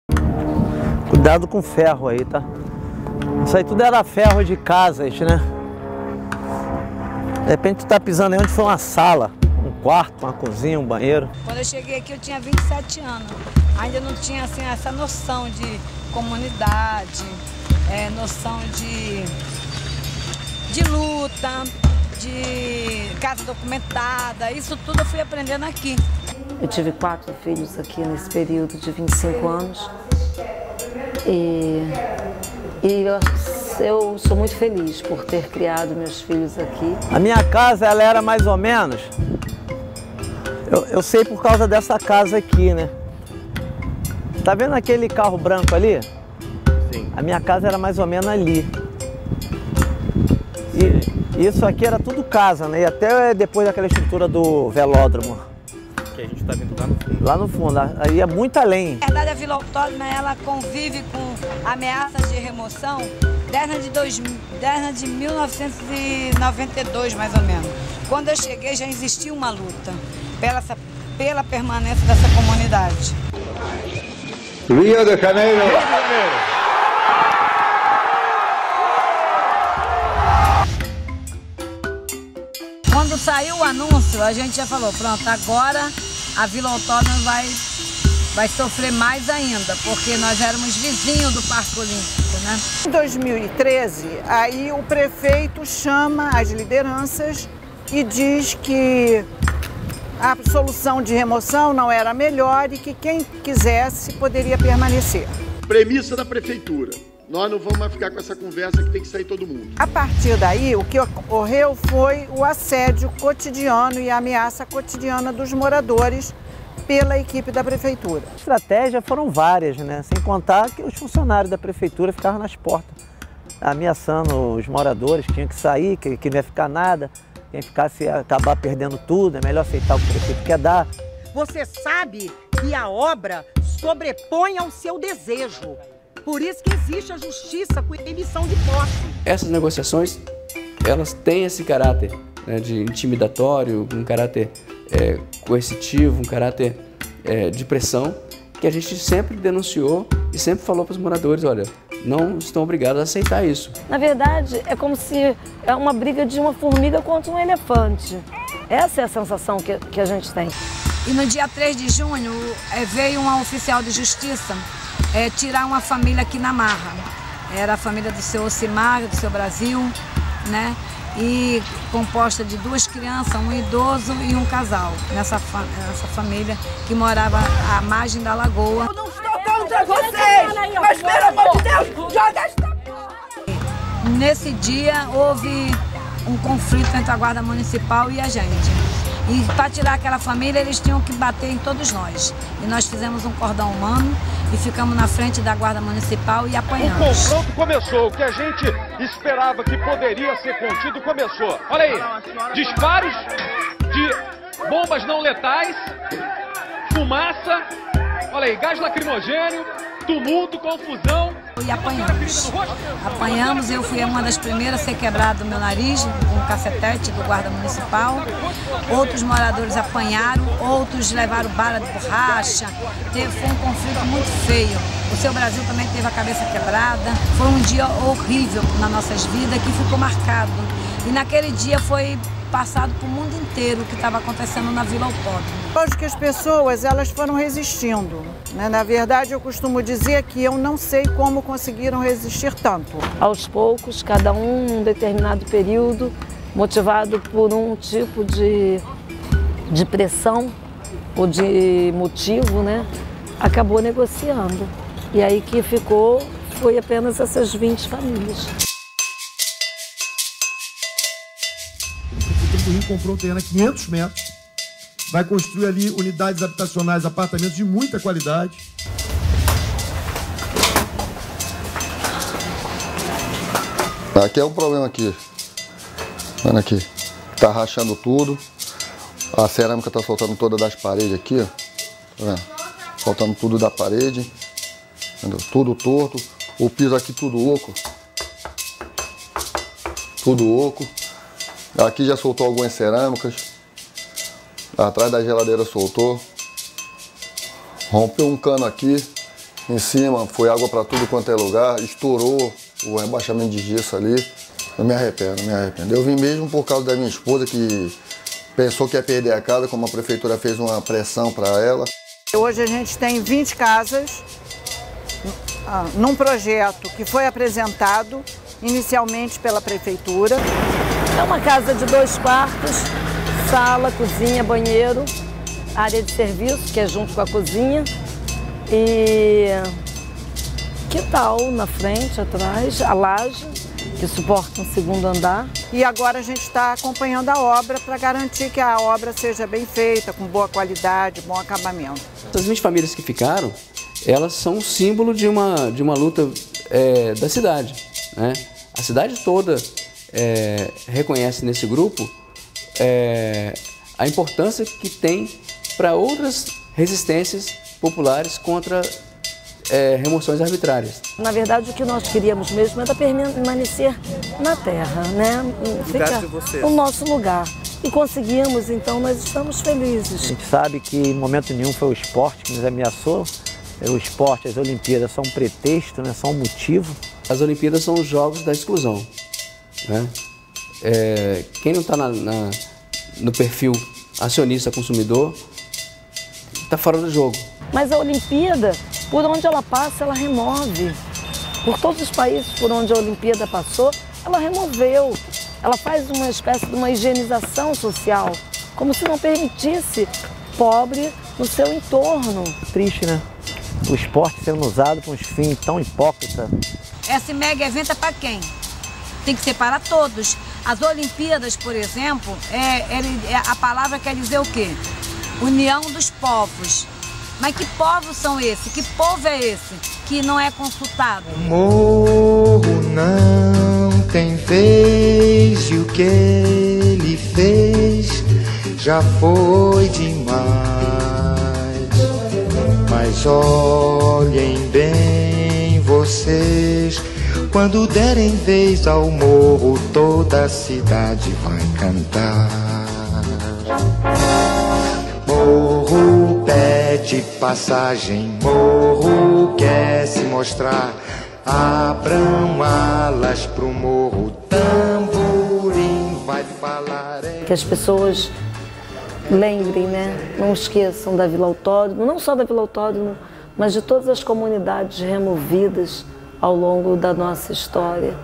Vila Autódromo: Medalha de ouro em resistência
Conheça a história da comunidade vizinha ao Parque Olímpico, no Rio de Janeiro, que resistiu a dezenas de desapropriações da Prefeitura. Vinte famílias contrariam a especulação imobiliária e permanecem vivendo na terra.